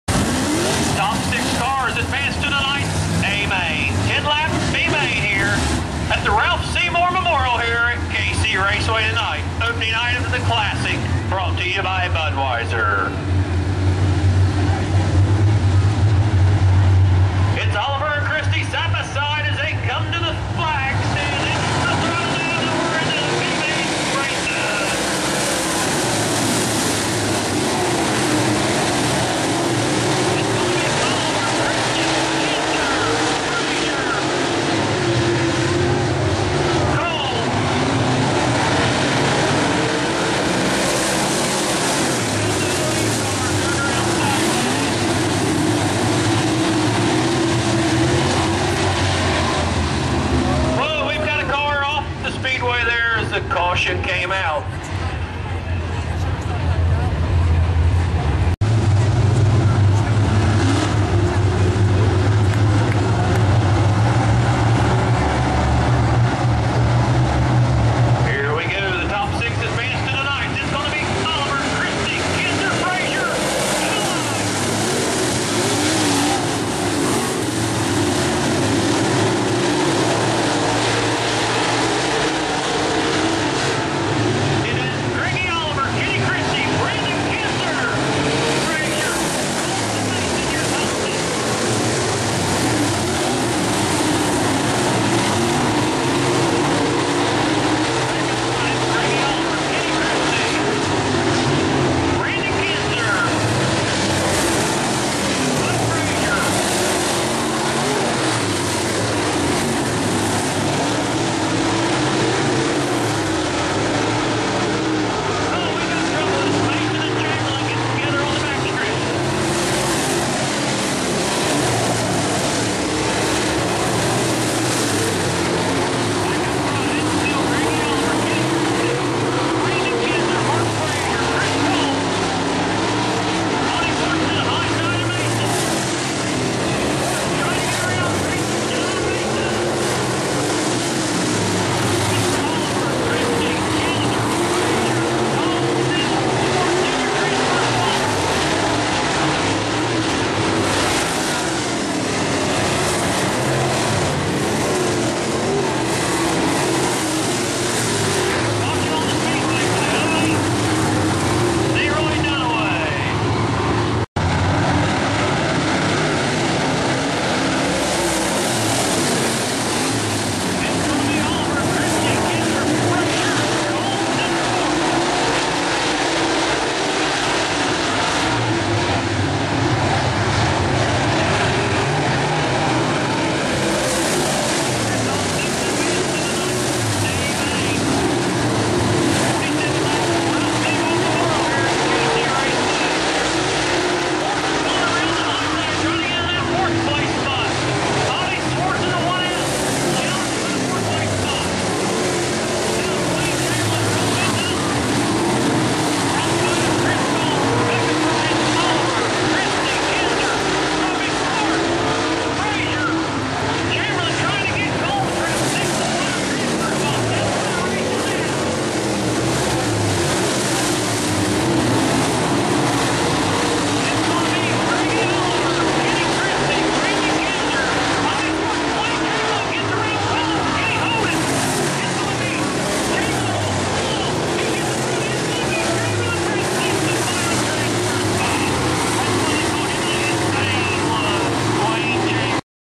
Ralph Seymour Memorial B-main
The b-main from the Ralph Seymour Memorial at K-C Raceway, 4-30-10.